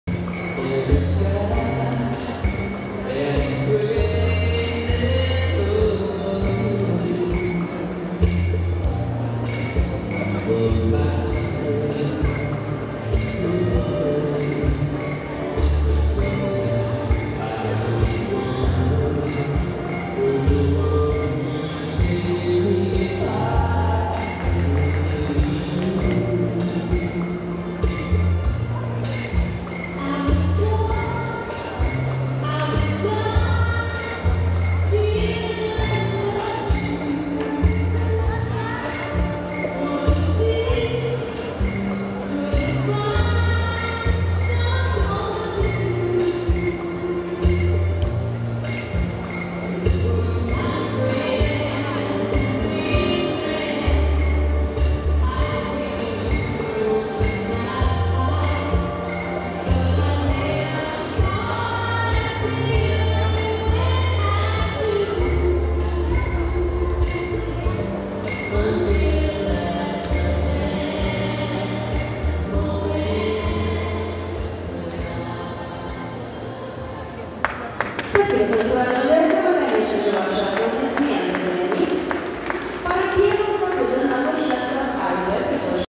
Мазуров Илья - Беломестнова Василина, 1/16, Rumba
Чемпионат и Первенства Российского Танцевального Союза 2016, Взрослые ЛА, InterDance.ru